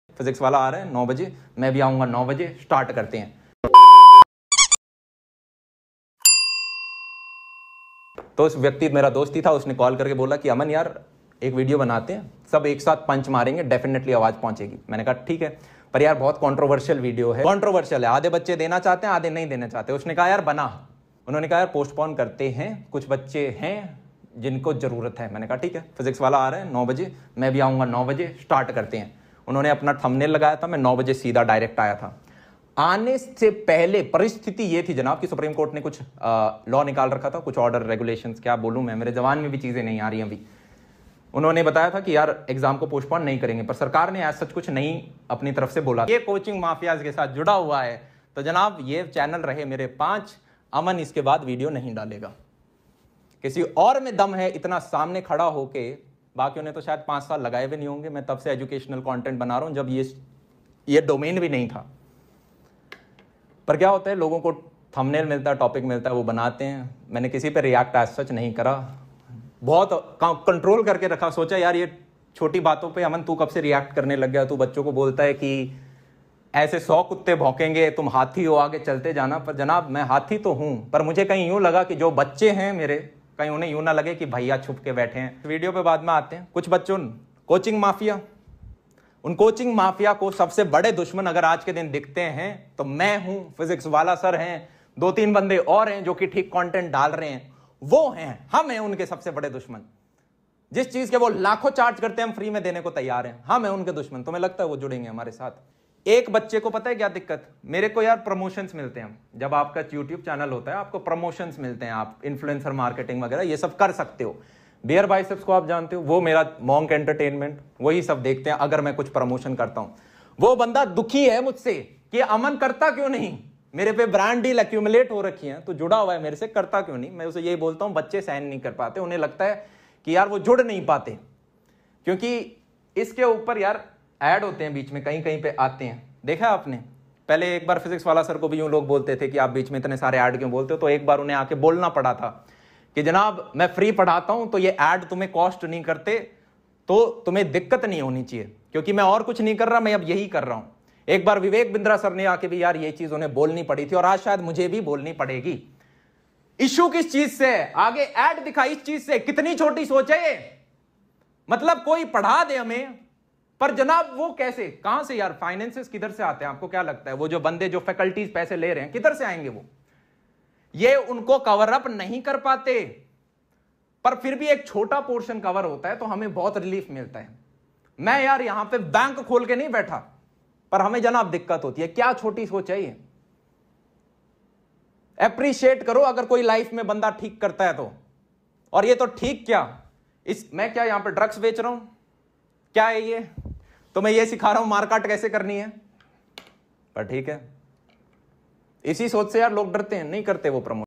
AMAN DATTARWAL Talking About PHYSICSWALLAH SIR
Tittle-

This video is basically a video of aman DATTARWAL in which he explain the video of jee neet postpone and talking about Physicswallah sir